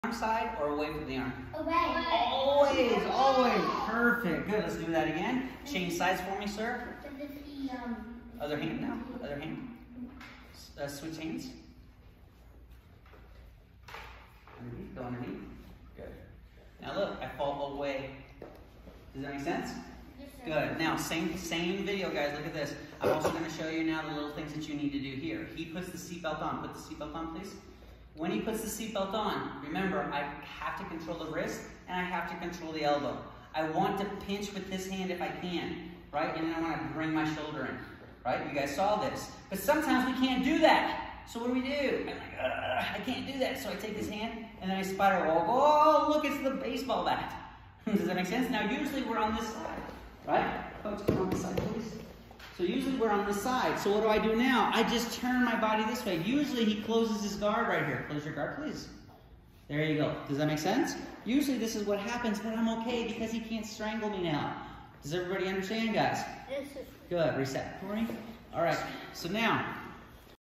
Back Defense
Falling to the correct side plus moving the arm to the opposite side (AKA - baseball bat escape)

Kids version